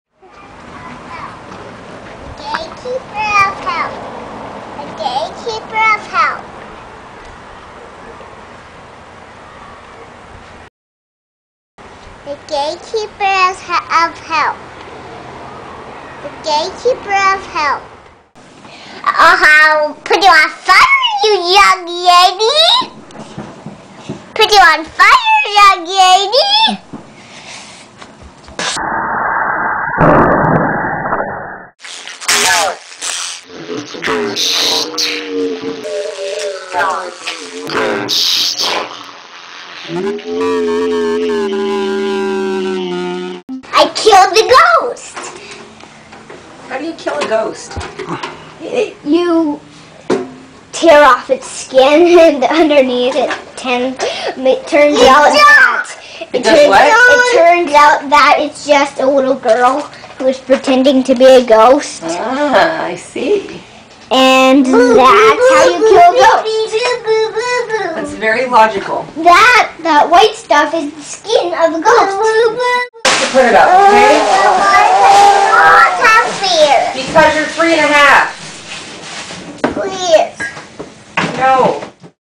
Lucy Kills A Ghost (A Jesus & The Robot Production)
Lucy explains how to kill a ghost. Simply detailing what paranormal experts have been unable to figure out since the start of time.